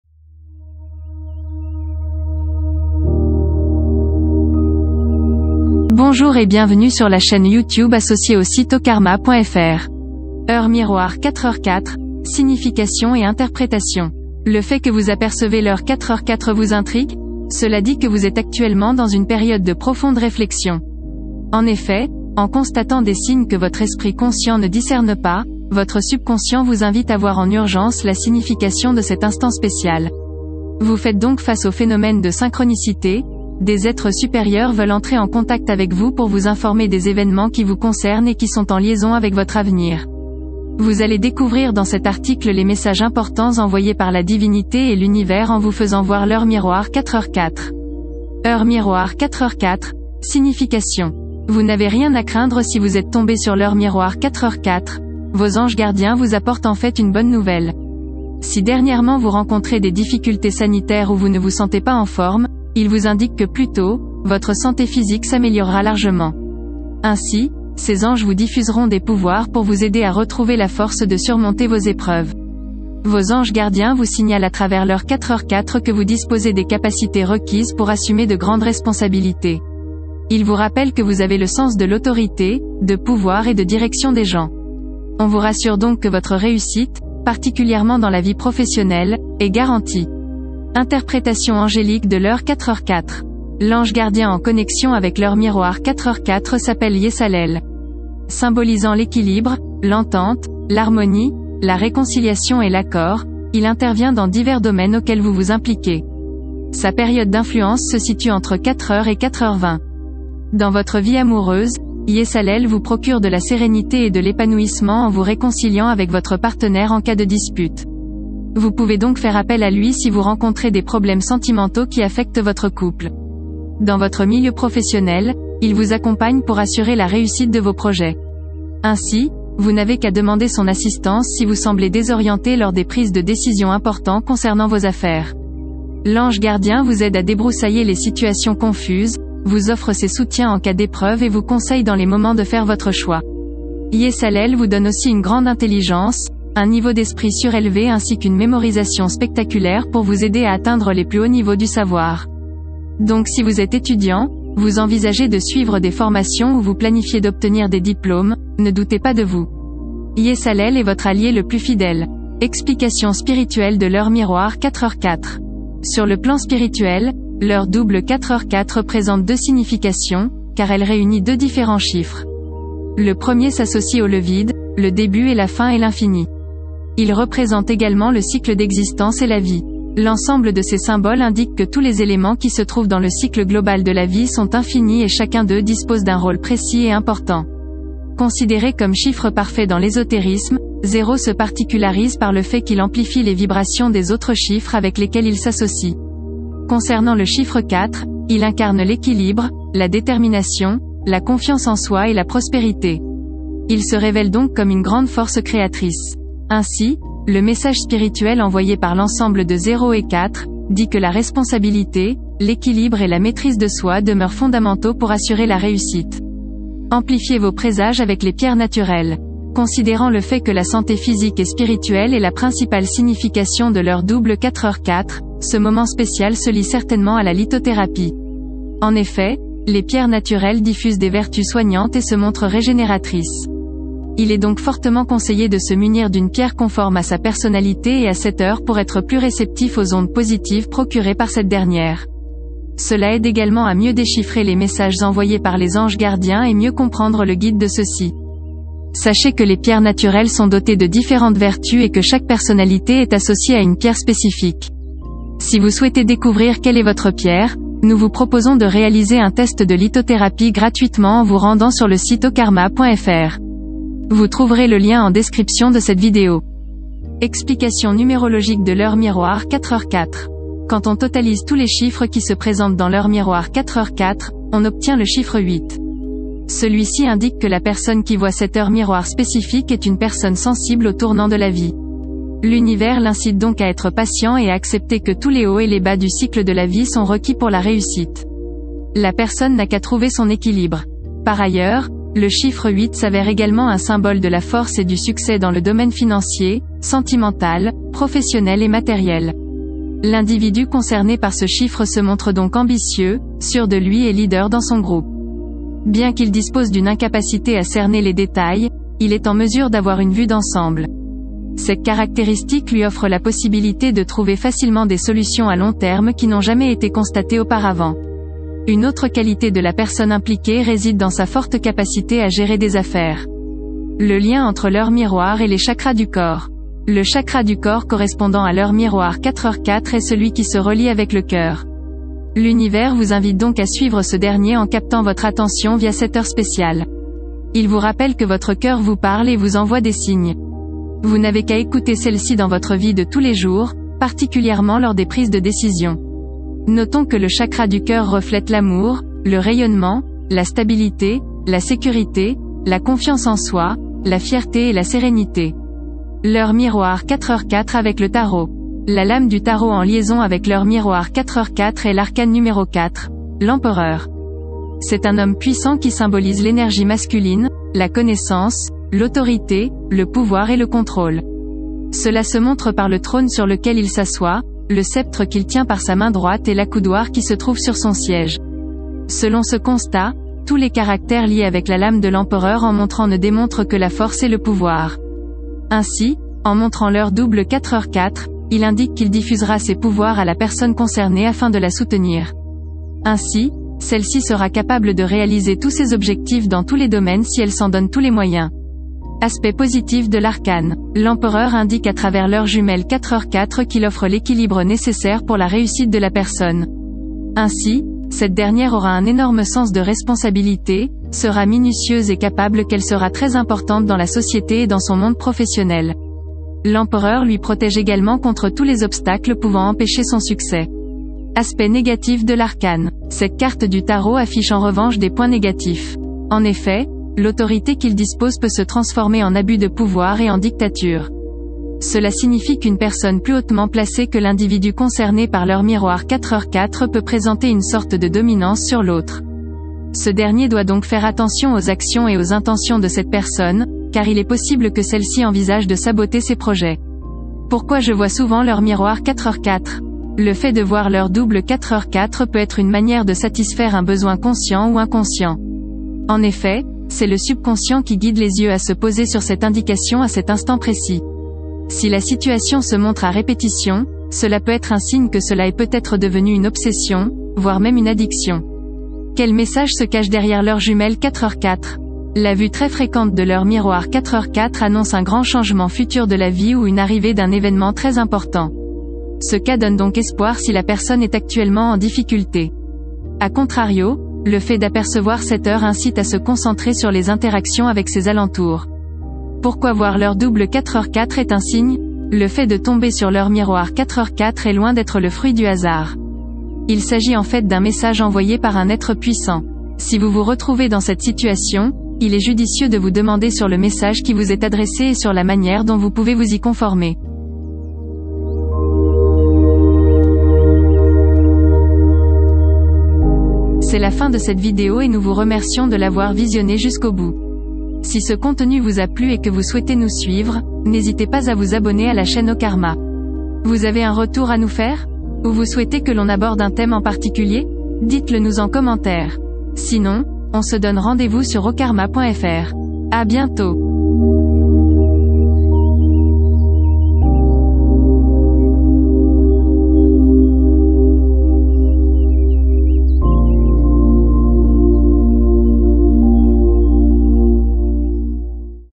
🕛 HEURE MIROIR 04h04 - Interprétation et Signification angélique
💎 Saviez-vous que le présage des heures miroirs pouvait être amplifié avec l'usage de pierre naturelles ?

Signification de l'heure miroir 04h04 : Explications des messages angéliques 👼

00:11 Heure miroir 04h04 : Signification et interprétation
00:50 Heure miroir 04h04 : Signification
01:32 Interprétation angélique de l’heure 04h04
02:43 Explication spirituelle de l’heure miroir 04h04
03:37 Amplifiez vos présages avec les pierres naturelles
04:28 Explication numérologique de l’heure miroir 04h04
05:25 Le lien entre heure miroir et les chakras du corps
05:57 L’heure miroir 04h04 avec le tarot
06:40 Aspect positif de l’arcane
07:02 Aspect négatif de l’arcane
07:28 Pourquoi je vois souvent l’heure miroir 04h04 ?
07:50 Quel message se cache derrière l’heure jumelle 04h04 ?
08:10 Pourquoi voir l’heure double 04h04 est un signe ?